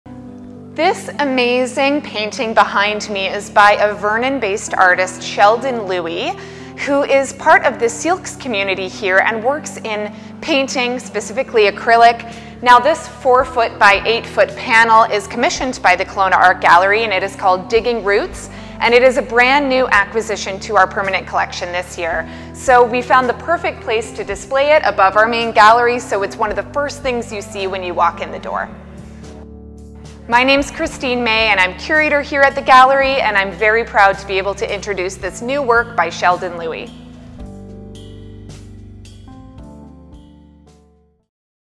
New commission by Sheldon Pierre Louis at the Kelowna Art Gallery
Digging Roots by Sheldon Pierre Louis
New commission by Sheldon Pierre Louis
July 1, 2023 – ongoing

Sheldon Pierre Louis, swicm – digging roots, 2023, 4 x 8 feet, acrylic on canvas

The Kelowna Art Gallery is pleased to announce this new commission by artist Sheldon Pierre Louis. Louis’ large-scale painting on canvas draws inspiration from his ancestral roots, building on his existing multidisciplinary art practice in painting, drawing, carving, and sculpting.

Sheldon Pierre Louis, a member of the Syilx Nation, is part of the Re-Think 150: Indigenous Truth Collective—a group of Indigenous and non-Indigenous allies working to educate society on Indigenous and environmental issues. Louis has previously exhibited at the Kelowna Art Gallery, Enowkin Centre, Lake Country Gallery, Arts Council of the Central Okanagan, The Rotary Centre for the Arts, Gallery Vertigo, and Vernon Public Art Gallery. His work is held in private collections across the globe, including in Australia and New Zealand. As a muralist, he has participated in the Nelson International Mural Festival, and created pieces in Downtown Kelowna, Central and Southern Okanagan Schools, as well within the communities of the Okanagan Nation.

You can visit the Kelowna Art Gallery online at: kelownaartgallery.com